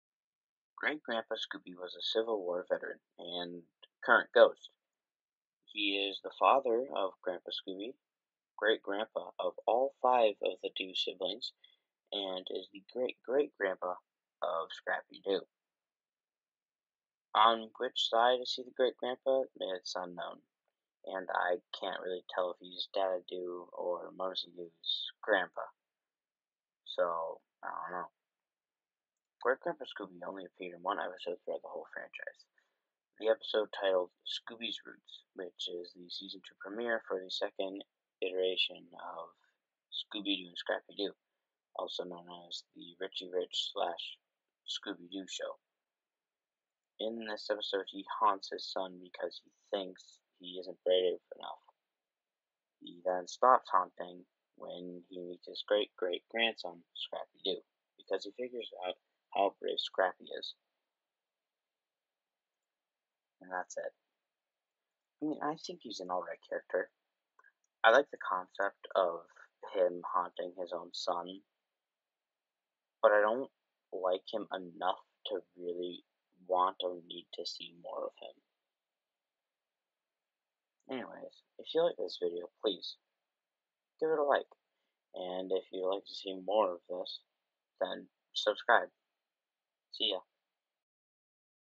The Great-Grandpa Scooby Character Archive
In this video I discuss Great-Grandpa Scooby's appearance in the Scooby-Doo franchise.